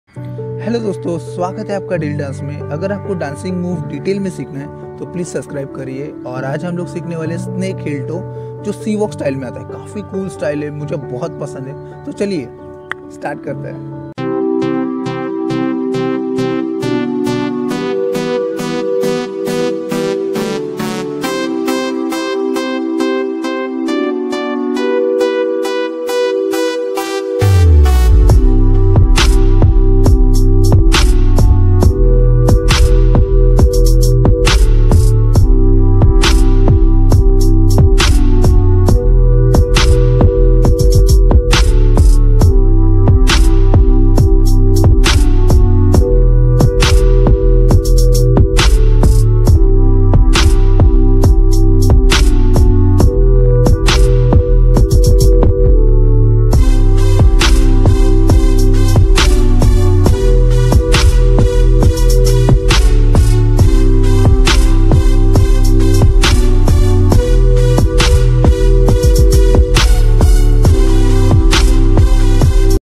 How To Do Snake Heel Toe ( C-walk Dance Tutorial For Beginner's) by Ajay Sakpal
Hip hop dance moves tutorial for beginner's